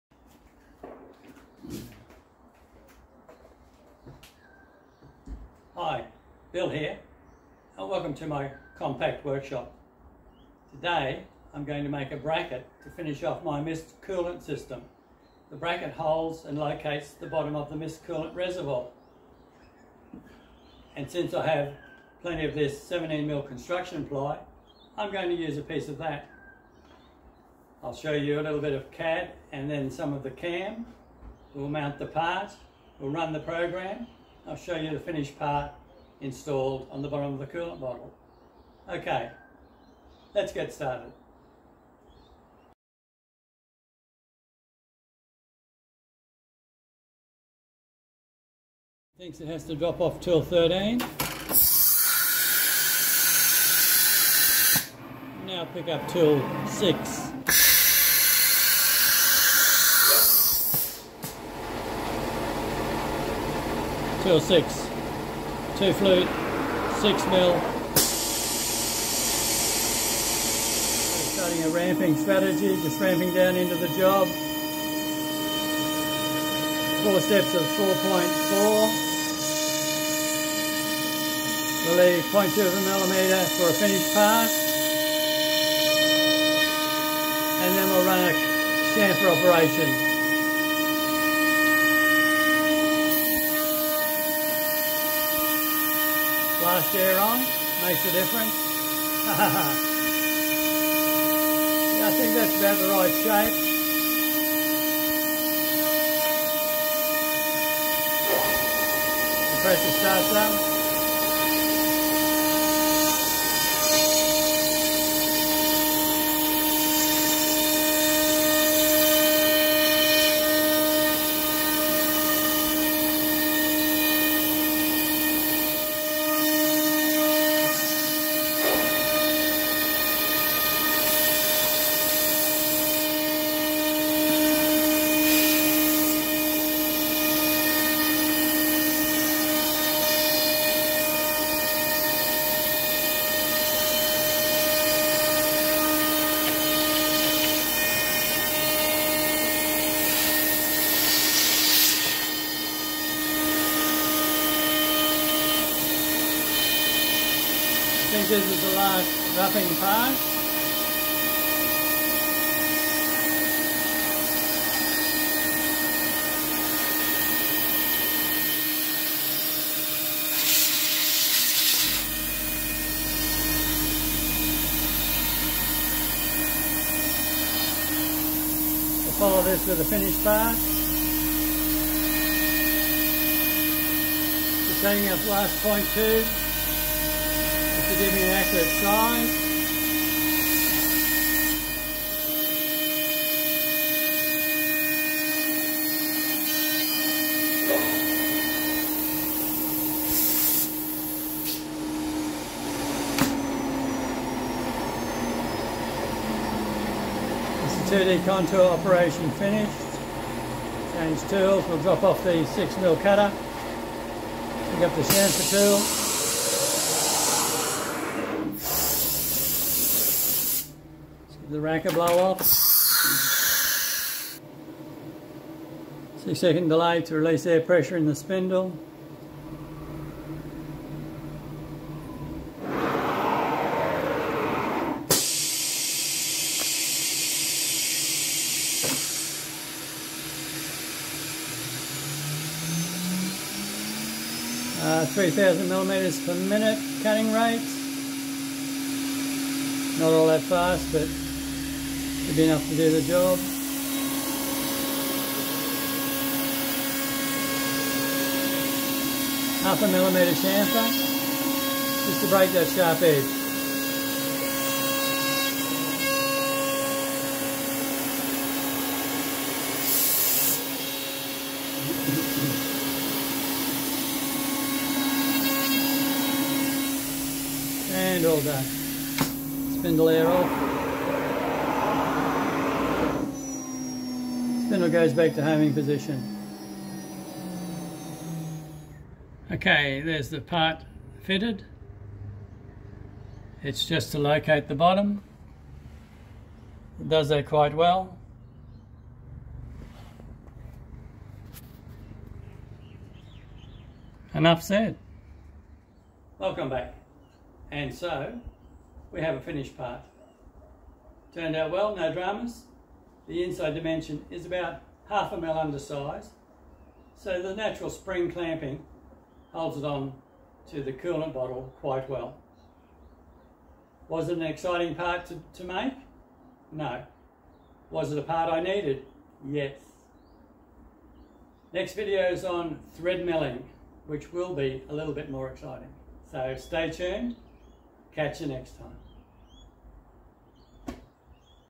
Bills PrintNC PRO + Tool Changer making a Fog Buster Coolant Bottle Mounting Bracket
To finish of the Fog Buster mist coolant system installation I needed a bracket to locate the bottom of the coolant bottle and so machined one out of 17mm construction ply on my home built PrintNC CNC Router with it's ATC and Tool Rack no big deal but the more times you run a program the better you get at it please feel free to comment cheers Bill